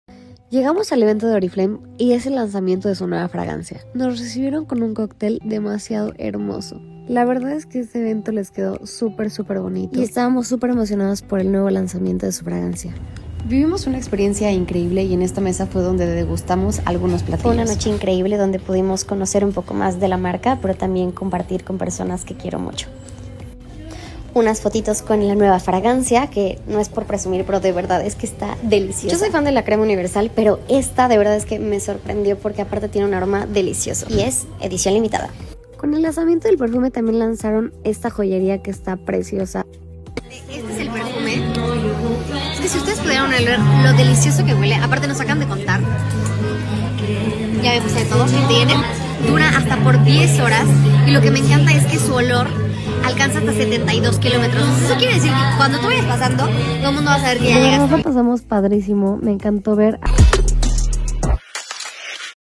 All or Nothing Amplified lanzamiento
Llena la habitación con tu presencia con la Nueva Fragancia *All or Nothing Amplified* :⁣
✨ Difusión de movimiento amplificada: un efecto de llenado de la habitación**⁣
✨Calidad duradera de 10 horas*⁣
✨ Absoluto de nardo y jengibre rojo
* OrpurTM de primera calidad.⁣
**Respaldado por una evaluación sensorial realizada por Givaudan que evalúa la intensidad percibida de la fragancia cuando alguien entra en la habitación.